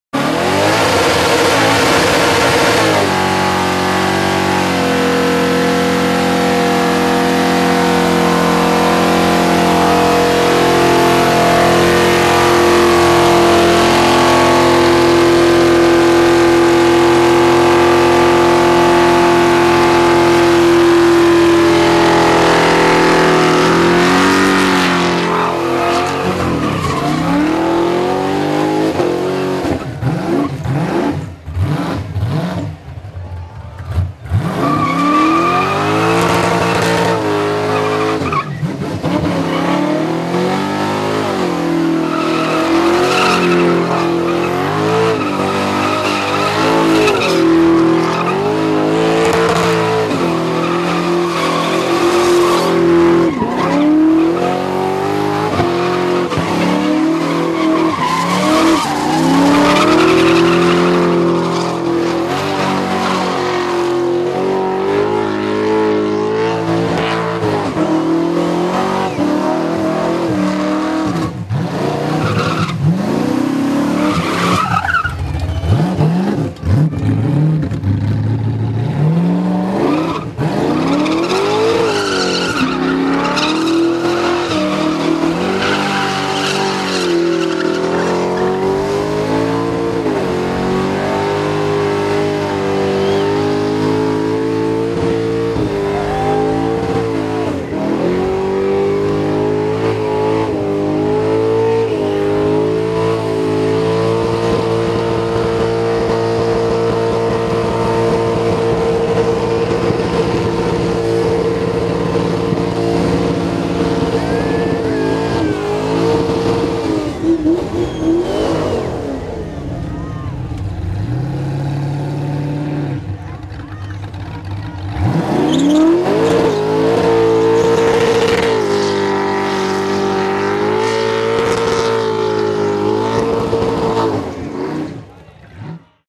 GMH HR Ute at Kandos
No wonder HR Holdens are one of the most popular of the early Holdens when they atre as tasty as this one. It's a had a bit of work done and by the sounds of things it's had a Chev V8 squeezed up front for some extra grunt.